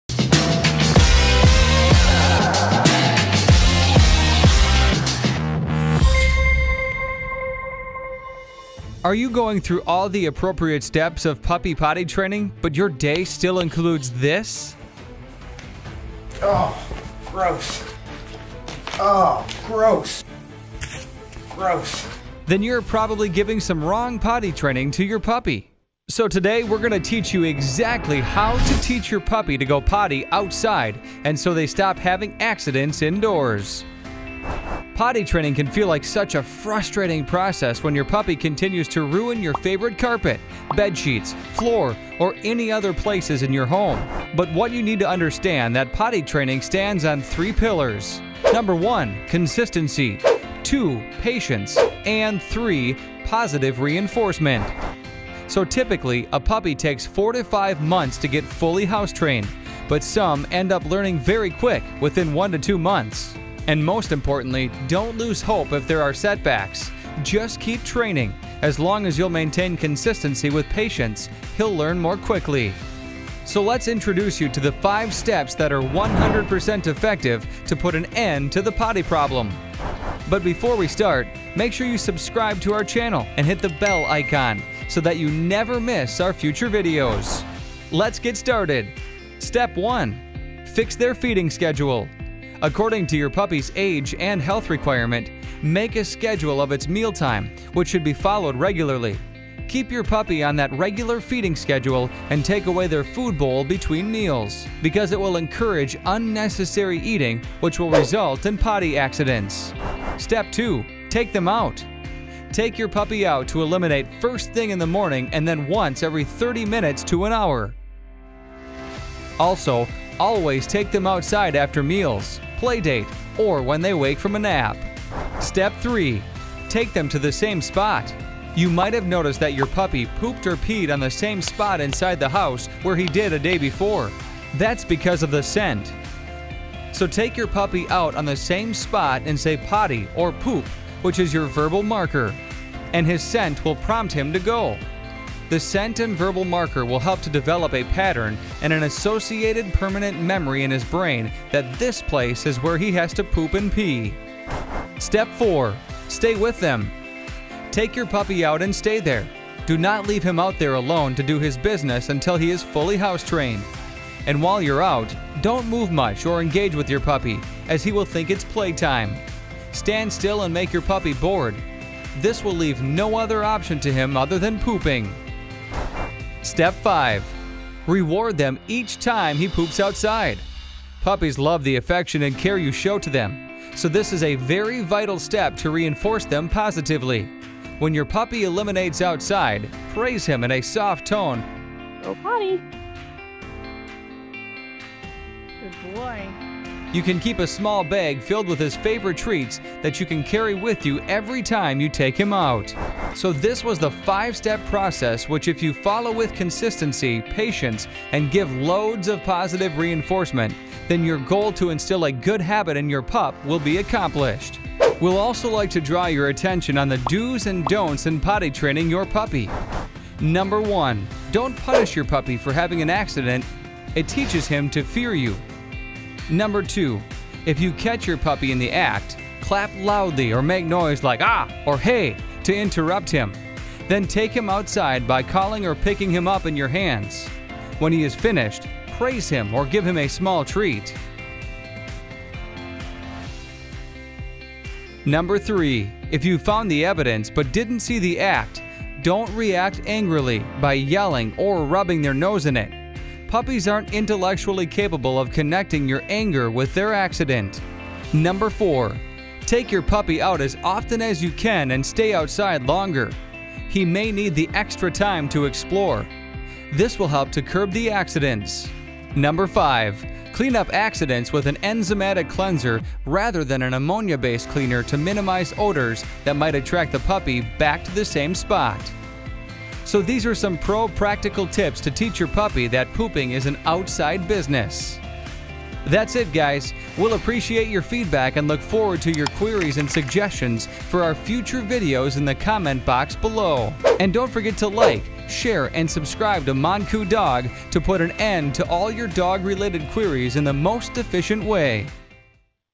Step by step guide To POTTY 💩 Train Your Puppy 🐶 easily. II Puppy Training tips. II
Are you going through all the appropriate steps of puppy potty training but your day still includes puppy poop and pee inside the house?
Then you’re probably giving some wrong potty training to your puppy.

Potty training can feel like such a frustrating process when your puppy continues to ruin your favorite carpet, bed sheets, floor, or any other places in your home. But what we need you to understand that potty training stands on 3 pillars.
1. Consistency
2. Patience
3. Positive Reinforcement

So, typically, a puppy takes 4-5 months to get full house trained but some end up learning very quick within 1-2 months. And most importantly, don’t lose hope if there are setbacks. Just keep training. As long as you’ll maintain consistency with patience, he’ll learn more quickly.

So let’s check out the 5 steps that are 100% effective to put an end to the potty problem.

We hope you'll like the video.
Please drop a comment if you have any great tips for us.